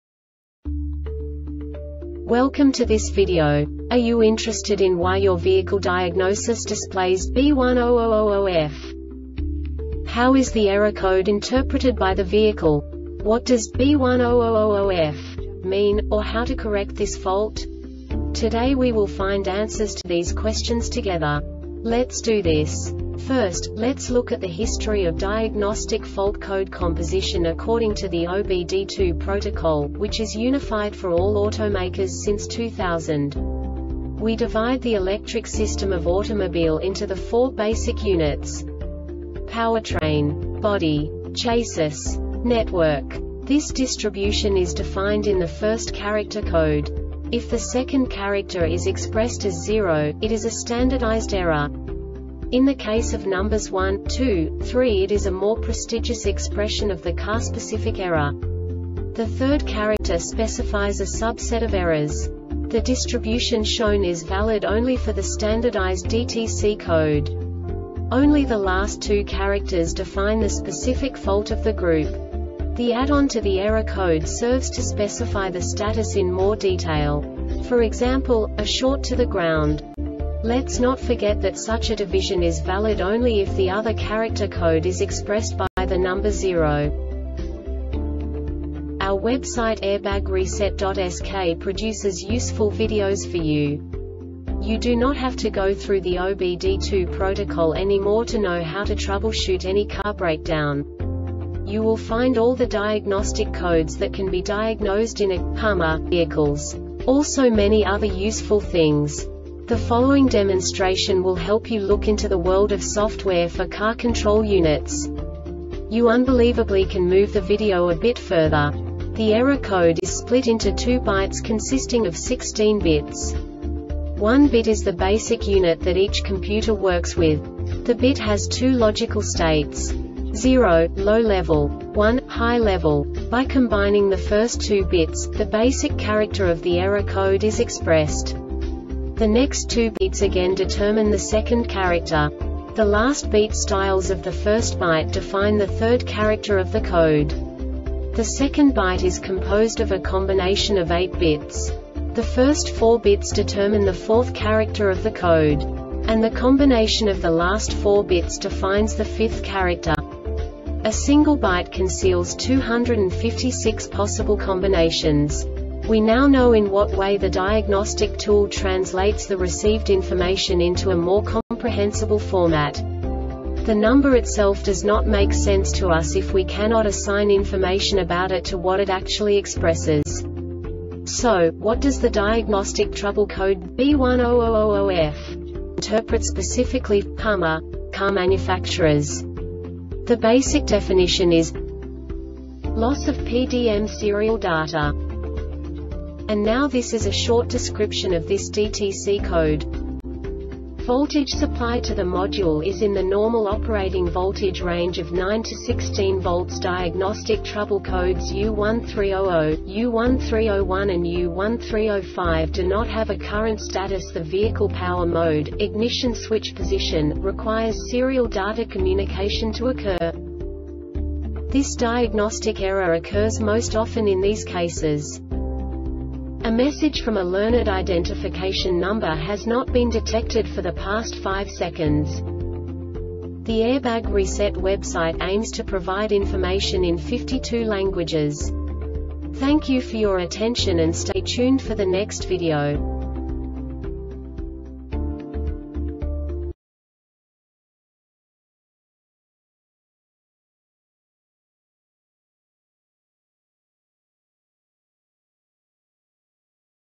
DTC Hummer B1000-0F Short Explanation
Contents:
0:21 Basic DTC analysis according to OBD2 protocol standard.
1:48 Insight into programming
2:58 A diagnostically understandable description of the Diagnostic Trouble Code
3:05 Basic error definition

Make:
HUMMER
Code:
B1000 0F
Definition:
Loss of PDM Serial Data
Description:
Voltage supplied to the module is in the normal operating voltage range of 9-16 volts.  Diagnostic trouble codes U1300, U1301 and U1305 do not have a current status.  The vehicle power mode,  ignition switch position,  requires serial data communication to occur.
Cause:
A message from a learned identification number has not been detected for the past 5 seconds.